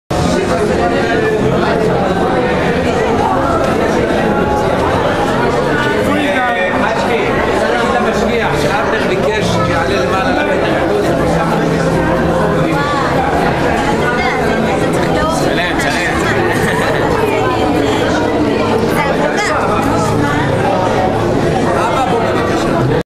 אריה טייב חתונה באולמי אזרעלי
אריה טייב מיתחתן ב 2012 עפם מוזמנים של 1000 איש